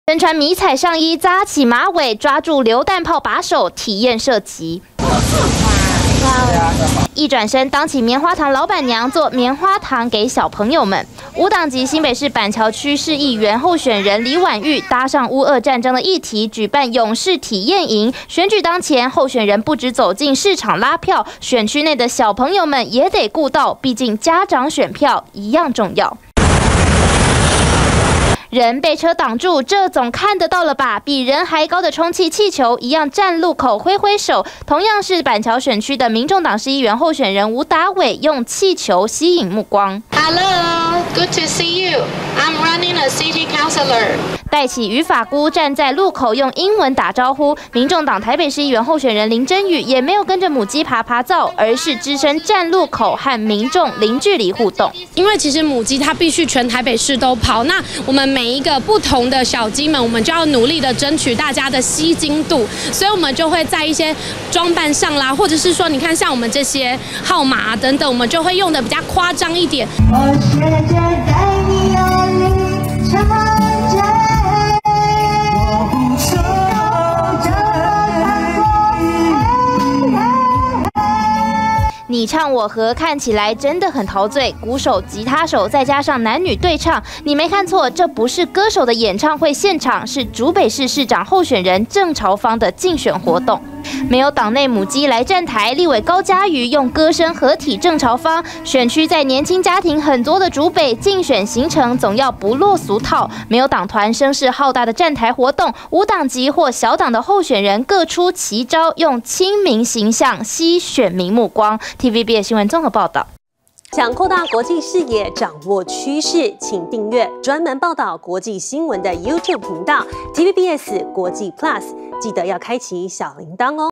沒母雞站台!候選人出奇招 爭「吸睛度」｜TVBS新聞
選舉進入倒數階段，大黨的候選人 有母雞可以靠，但是小黨或無黨籍的候選人 就得發揮創意。像是用氣球在路口揮手，吸引目光，還有候選人乾脆辦一場 戶外演唱會。